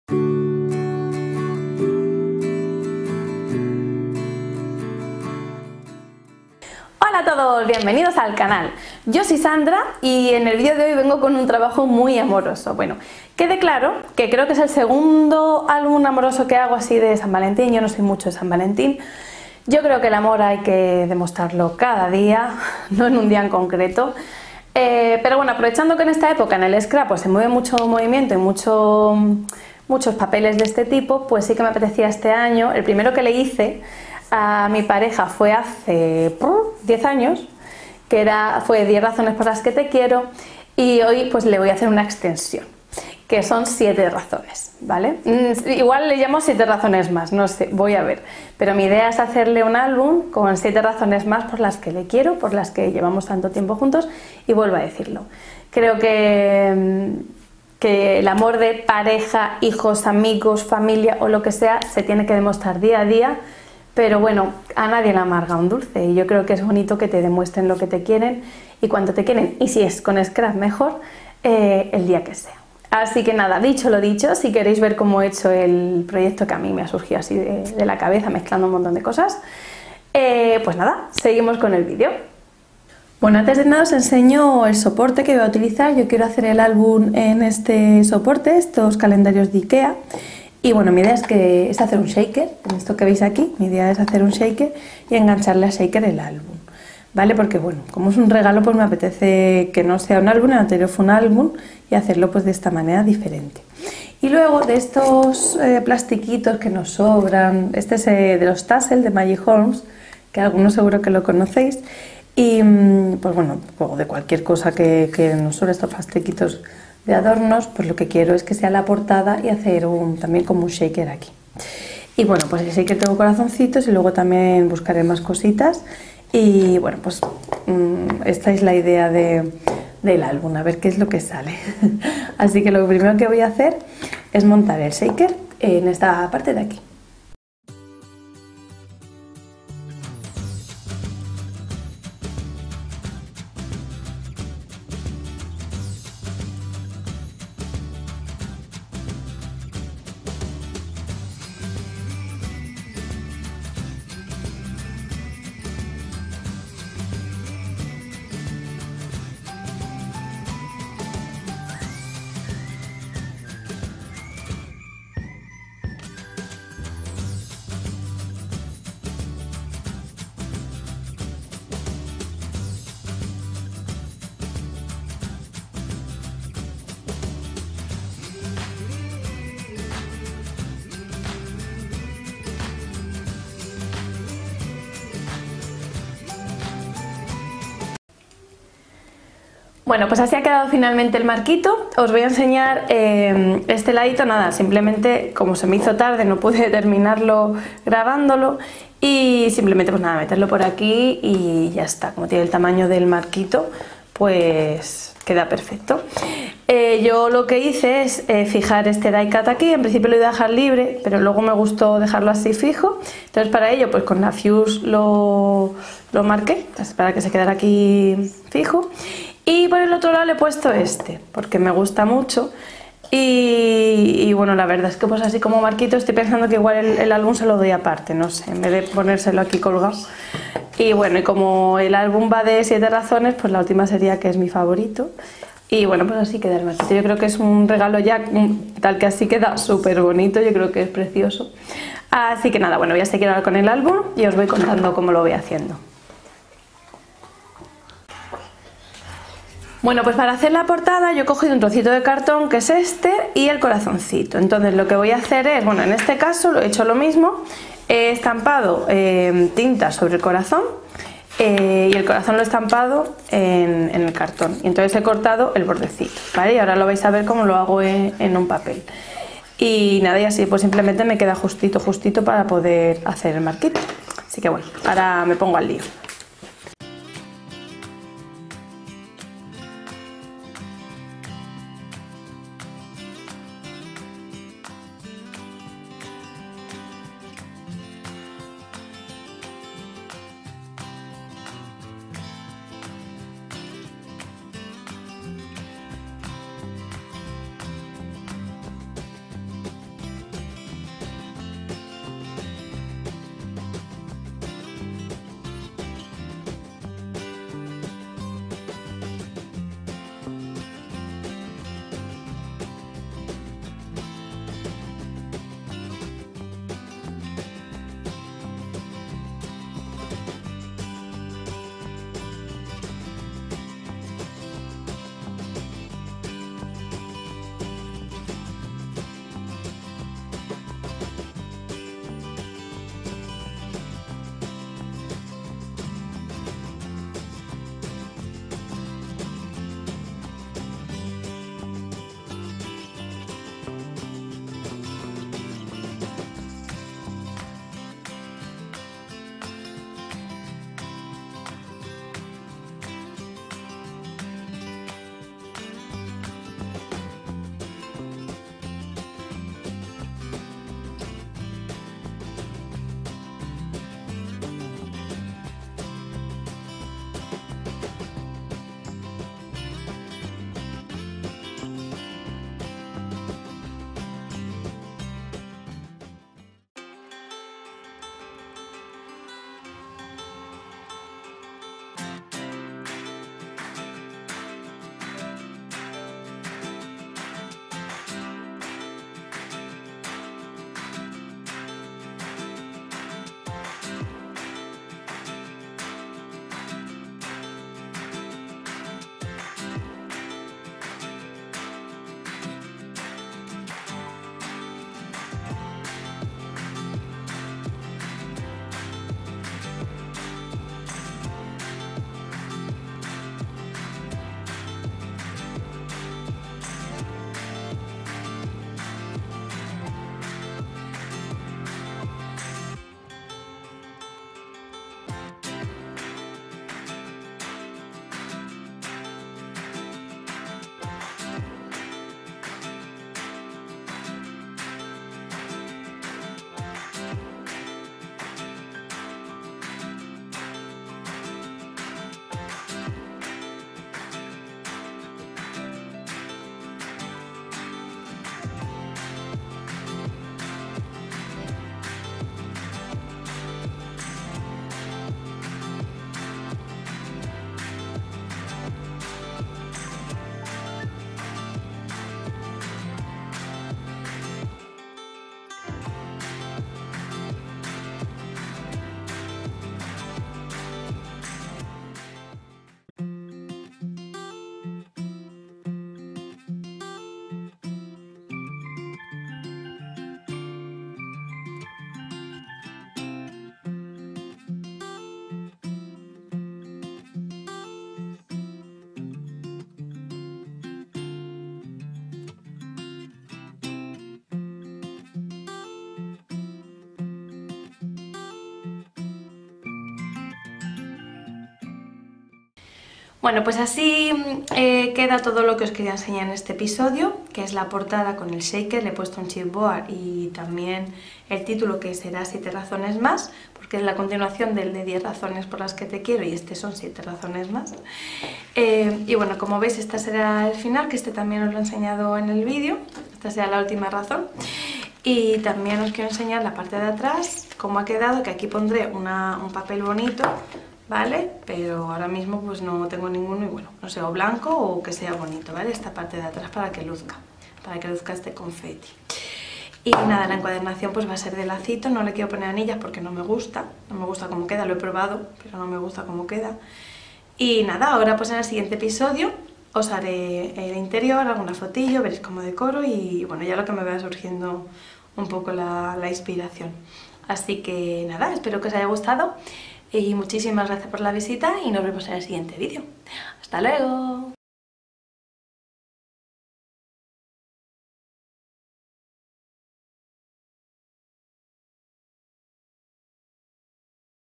Tutorial: Miniálbum La La Love Episodio 1 - por Los mundos de Sand
¡Se acerca San Valentín! Tanto si eres de l@s que celebran este día como si no, porque en el fondo cualquier día es bueno para dar cariño a las personas que queremos y si es con scrap mejor, este tutorial te va a encantar. Vamos a hacer un mini álbum muy romántico que es perfecto para regalar en este día tan especial.

En este primer episodio Sandra nos enseña como ha realizado la portada del miniálbum que va a regalar y el soporte en el que va montado, todo con la preciosa colección La La Love de Crate Paper, que podéis comprar en nuestra web

Va a usar también un marco de Ikea (concretamente el modelo Tolsby, por si queréis haceros con él), y algo que a priori solemos tirar, pero que ya veis lo genial que queda: el packaging de plástico en el que suelen venir muchas veces nuestros die cuts y adornitos. En este caso, está usando el de los archiconocidos Tassels de la colección Flourish de Maggie Holmes que tenía forma de corazón.

No os perdáis el siguiente episodio en el que lo decoraremos por dentro!

Si queréis las mismas cajitas redondas para almacenar vuestras lentejuelas (que sé que sí), son estas

Y a Los mundos de Sand!